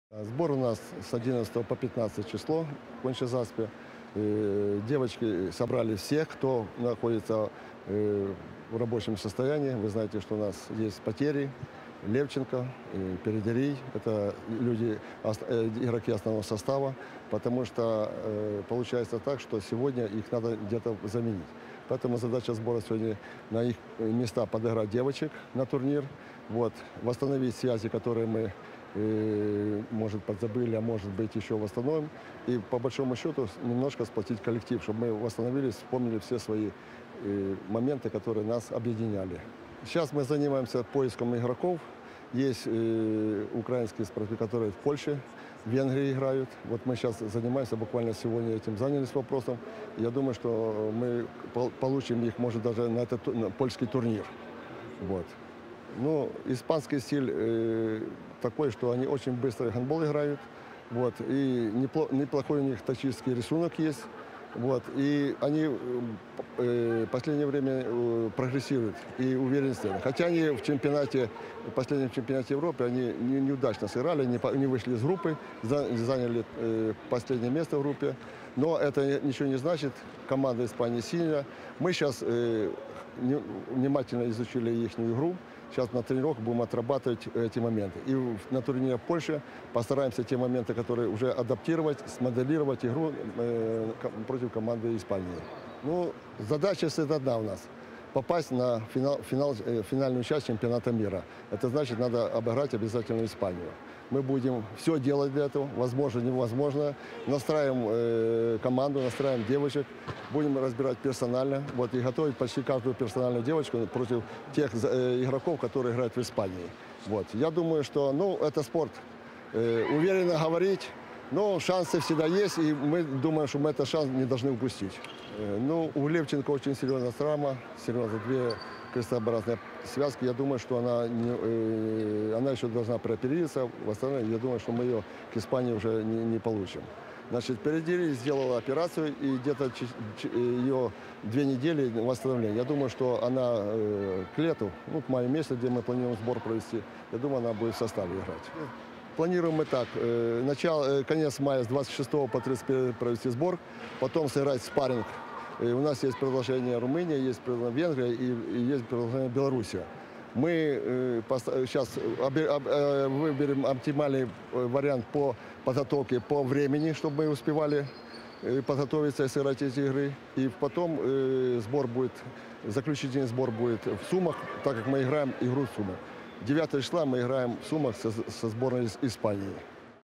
Борис Чижов - тренер женской сборной Украины по гандболу. О подготовке к матчам
Интервью с Борисом Чижовым тренером женской сборной Украины по гандболу. О подготовке к матчам